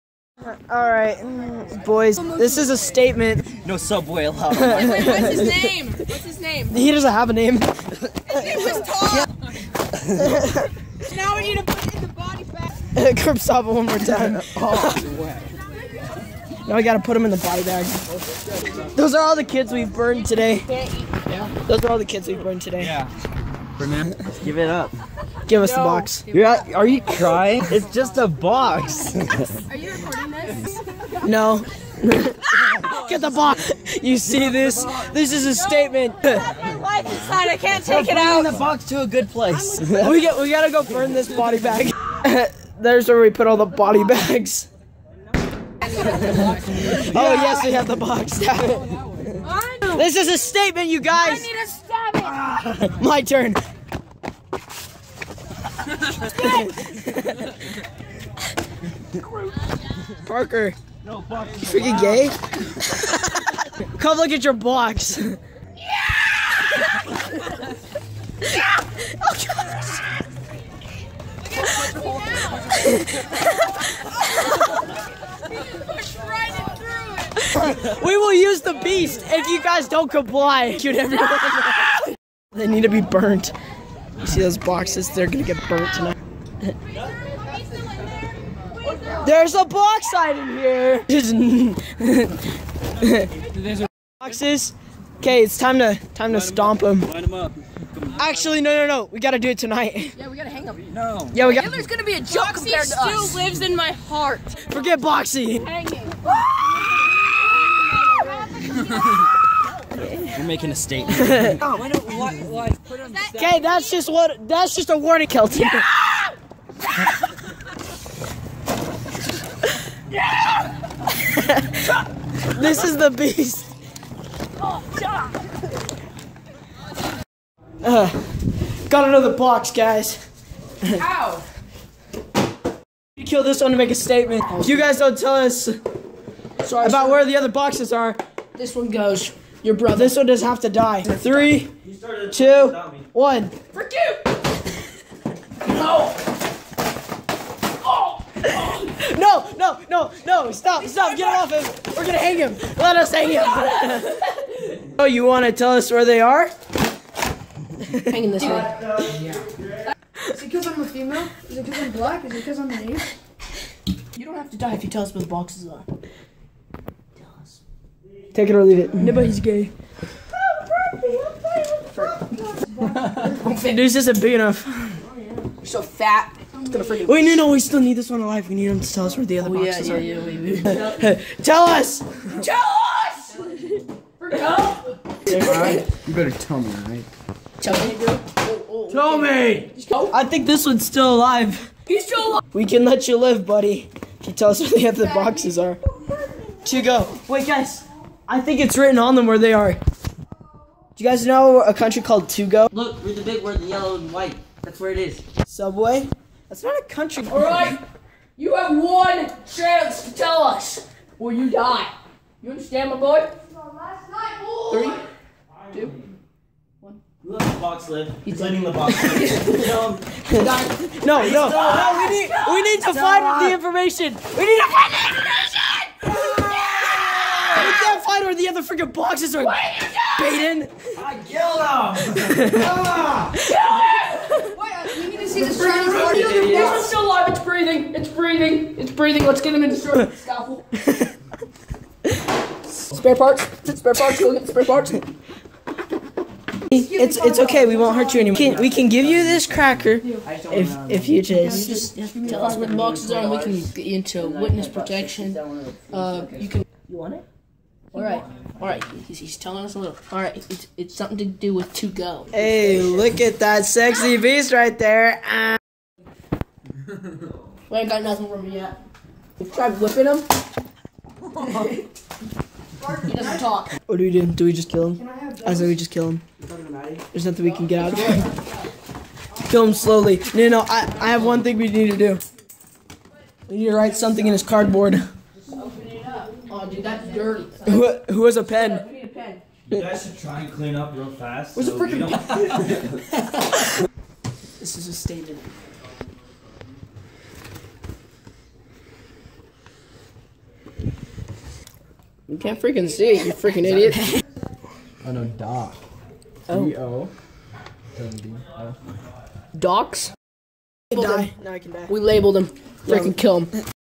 Box Massacre
This is a statement to show that the boxes cannot live. They are a threat to us and they just can't live because then... we would die or something. I don't really know where I'm going with this